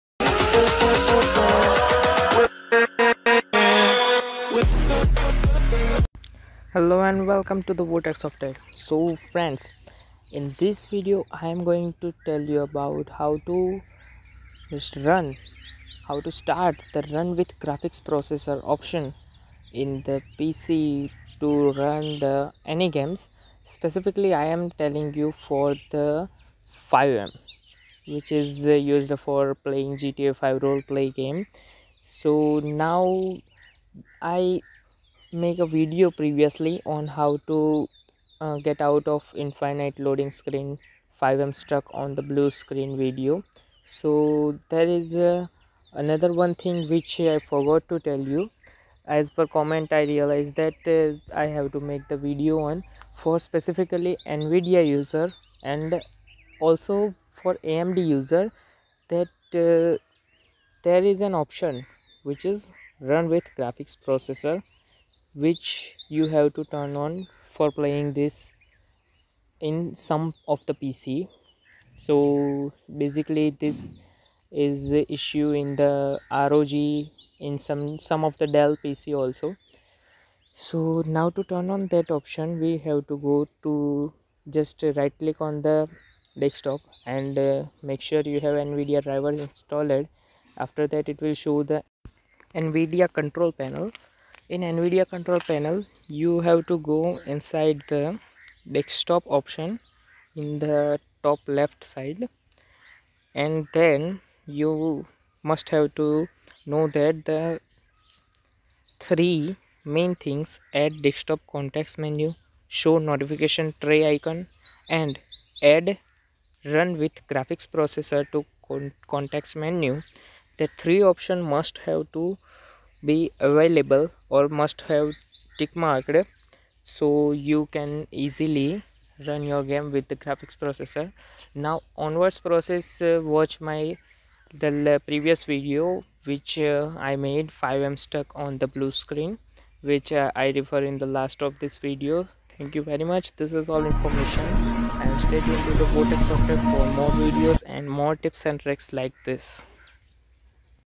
How to turn on run with graphics processor [FiveM problem Solution]
This video is another part of the Five M problem solution.

Five m which is the server application for the Gta 5 role play game.

Also you have to watch my another video.